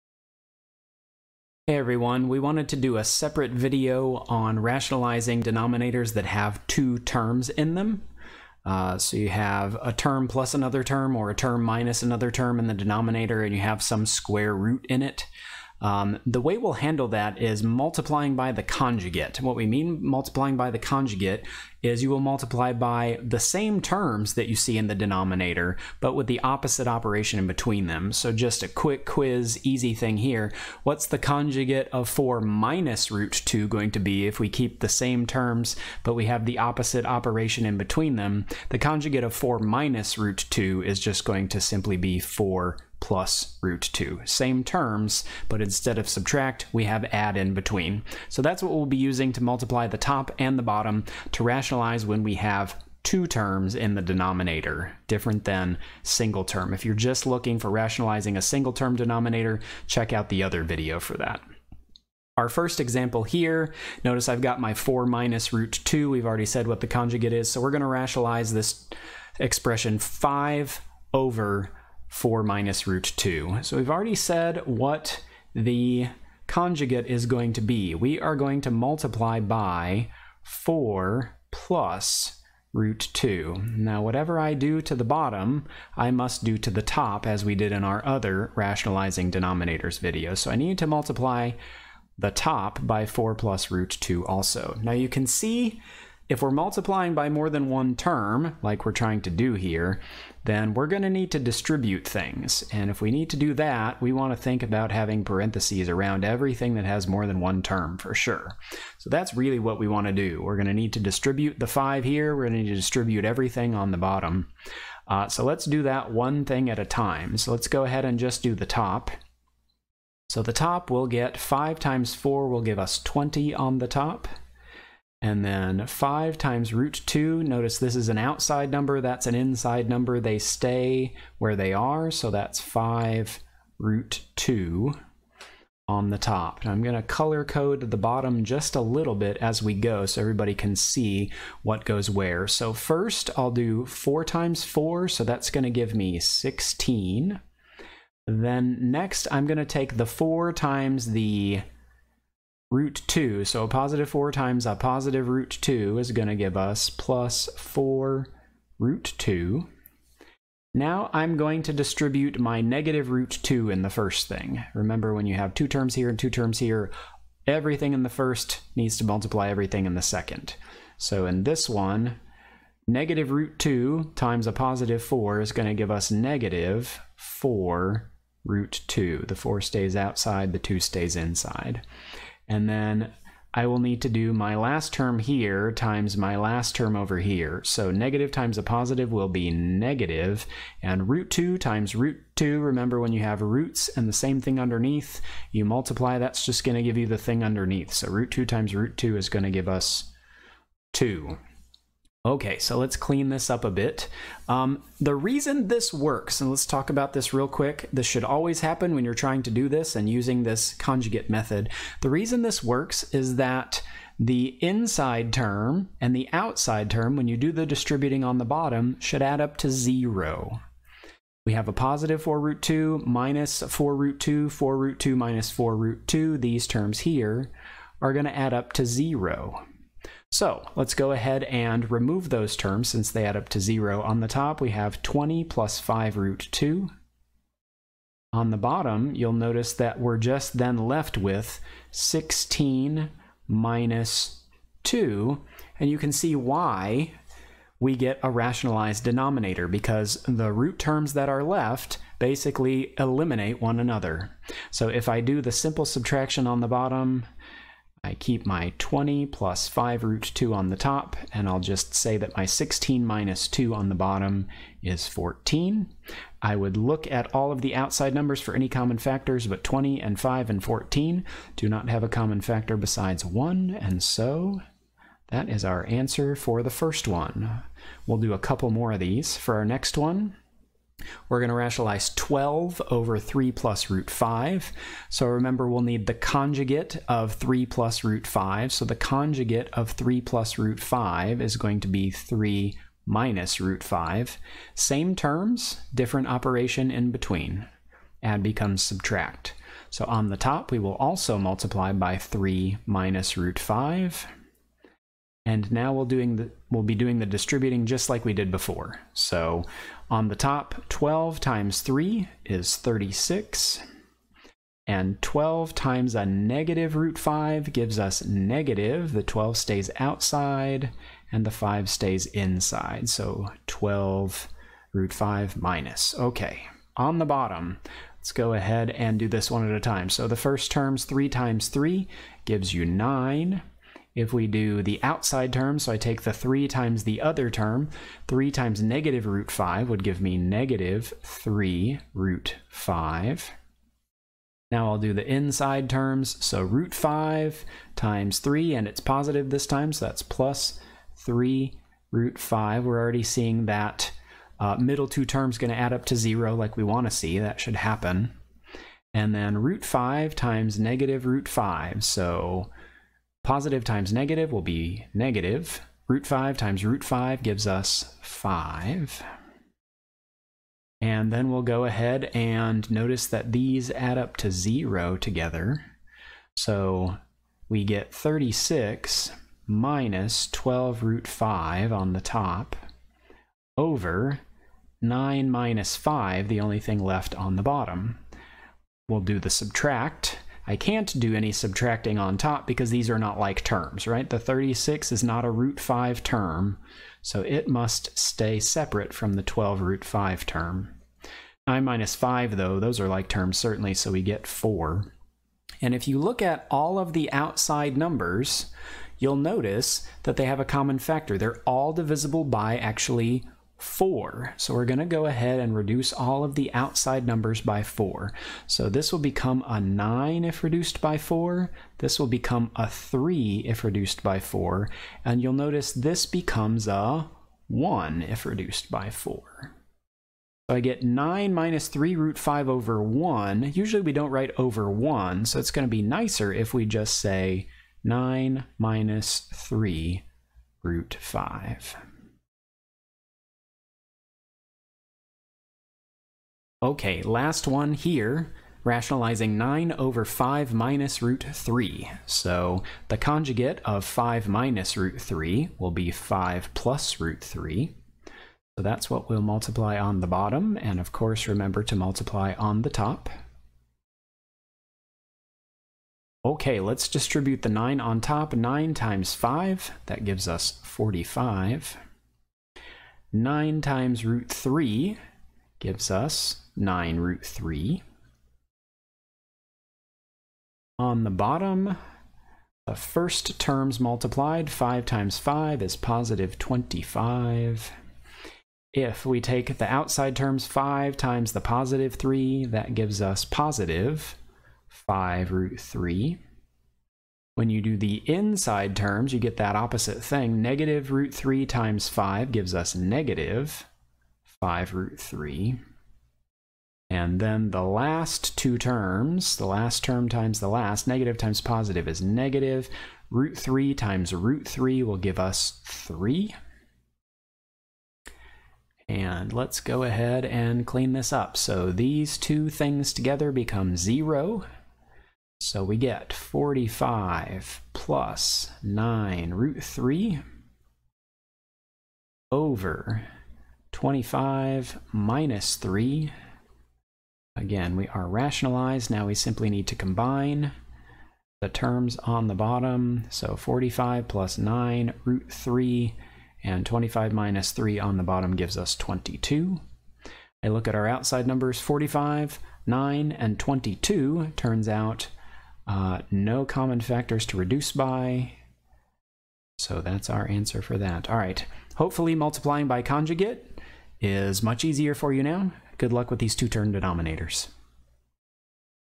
Rationalizing Denominators with Two Terms
Algebra tutorial video that explains how to rationalize a denominator with two terms by multiplying by the conjugate.  Examples shown.